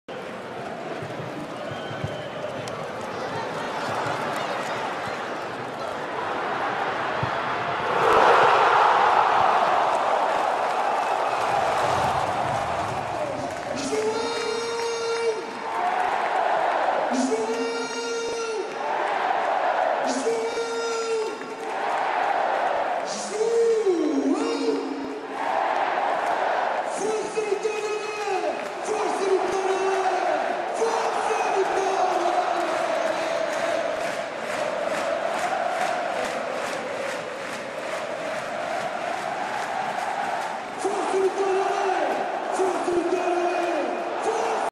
Golo João Mendes: Vitória SC (2)-0 FC Vizela - Liga Portugal Betclic | SPORT TV
O passe de Bustamante não sai, André Silva recupera e lança João Mendes que foge á defesa adversária e atira para o 2-0!